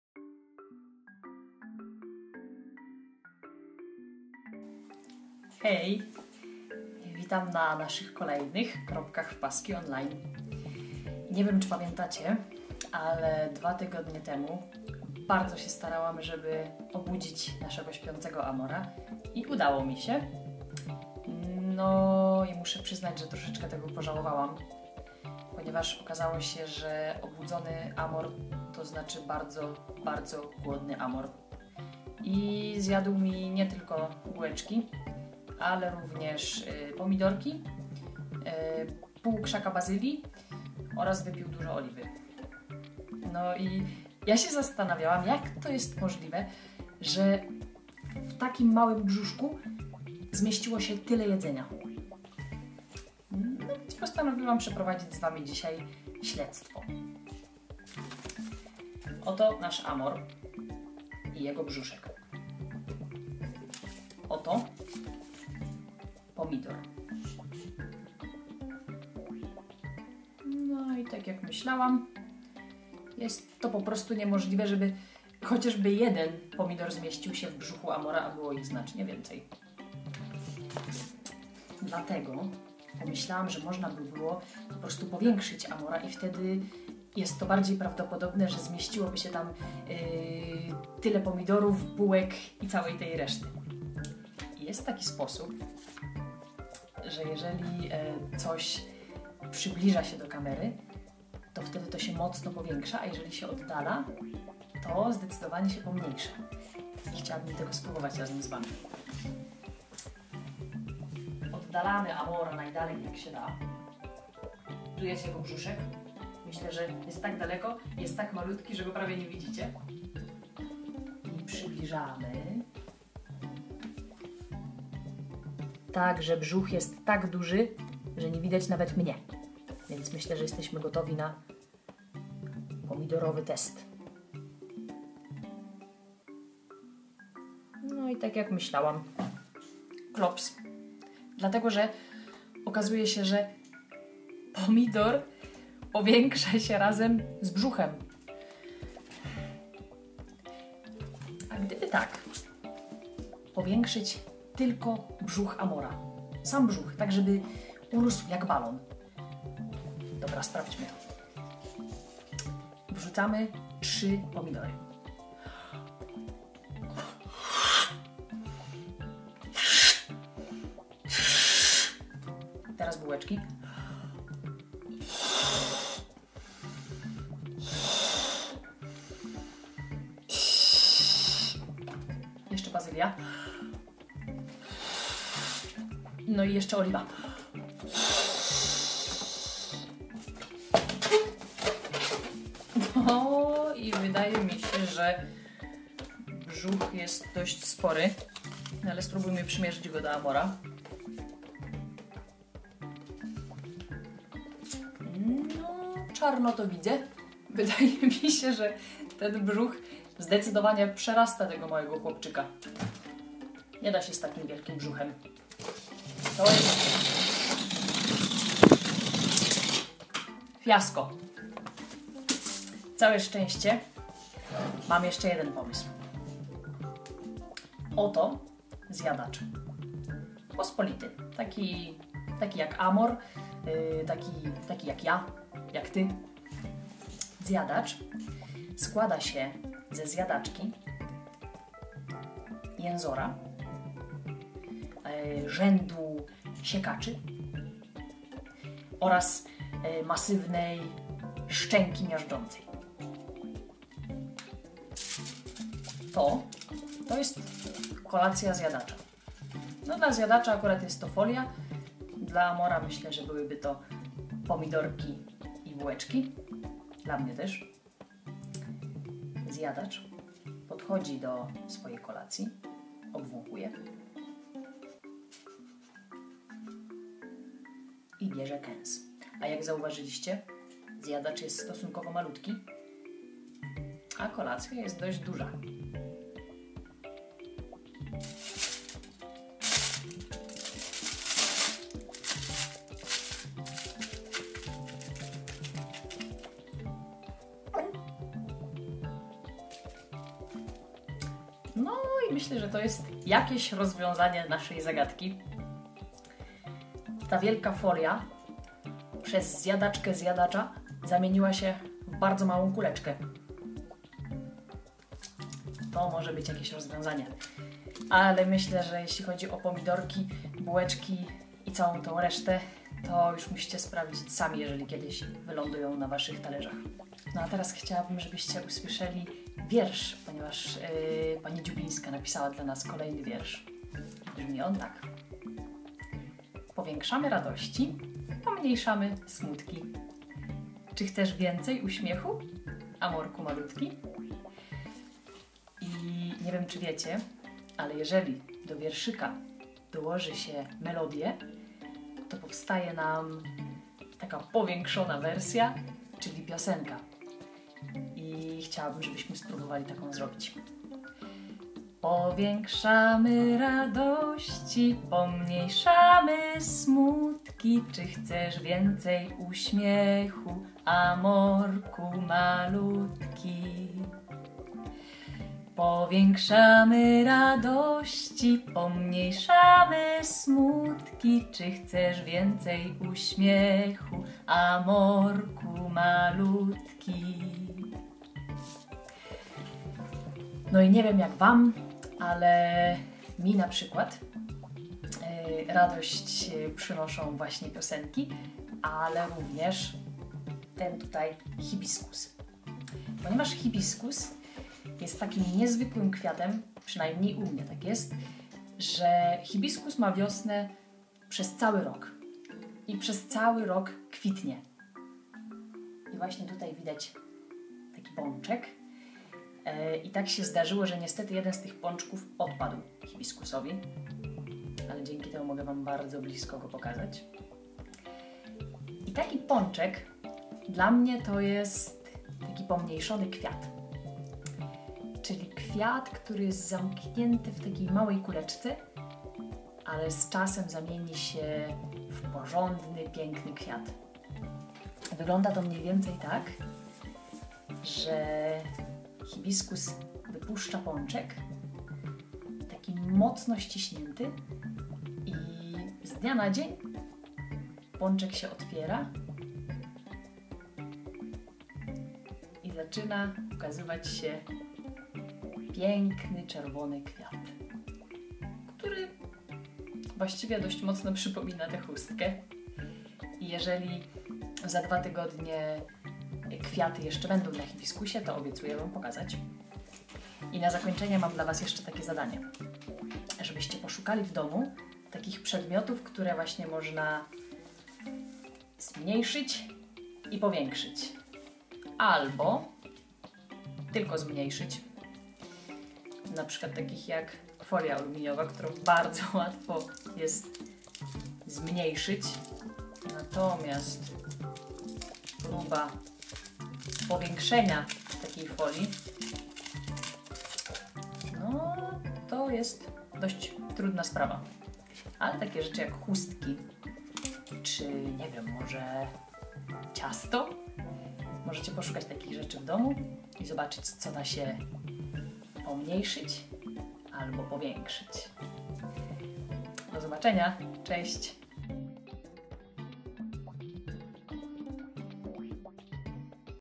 "Kropki w paski" online cz. II
Każdy śpioszek, gdy wstaje rano i po przebudzeniu myśli o jedzeniu, może mieć małe smutki i duży głód. Znajdźmy na to sposób!
Zapraszamy na kolejne spotkanie z cyklu Kropki w paski on-line, podczas którego opowiemy o wielkich i małych sprawach łazienkowskiego Amora.